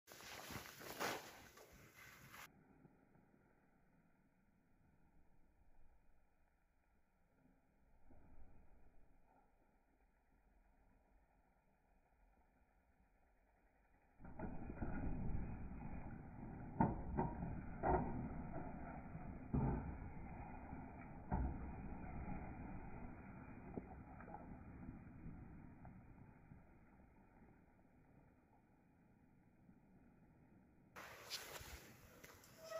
snow hopping
Groomed fatbike trails near Loud Performance in Stockton NY.